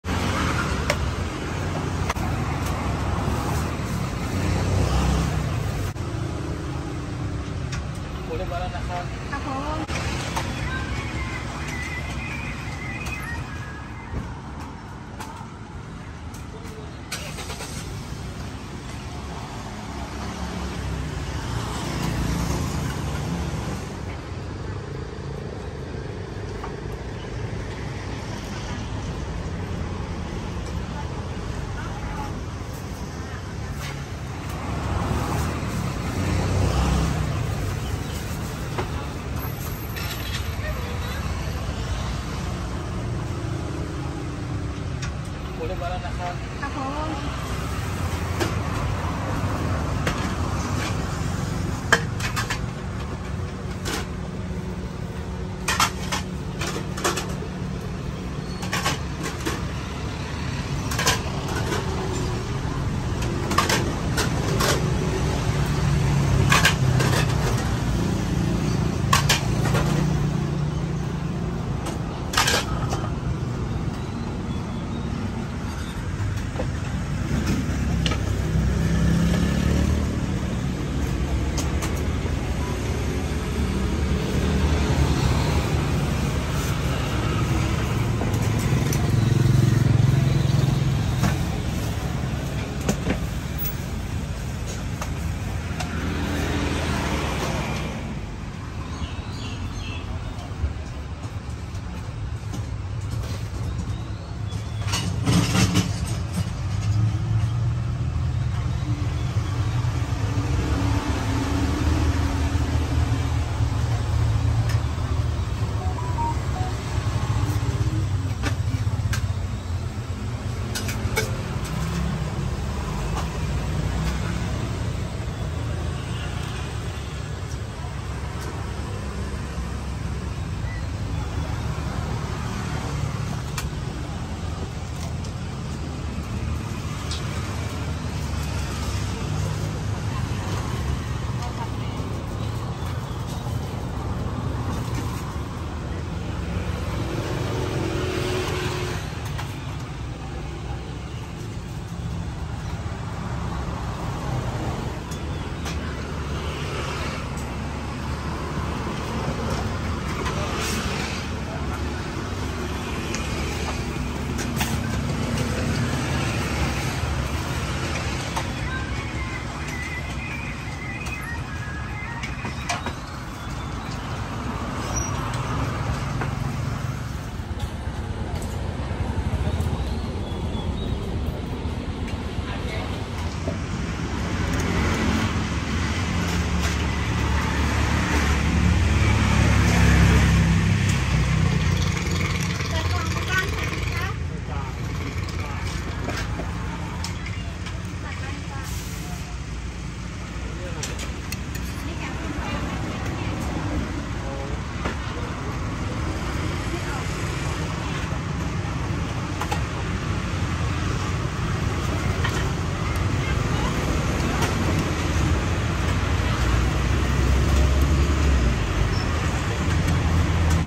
I Was Surprised , Local Thai Food with More then 30Menus [4K]
lt's a very tasty and interesting dish.
i eat for 2 people only 4.50$ , i was chocked by the price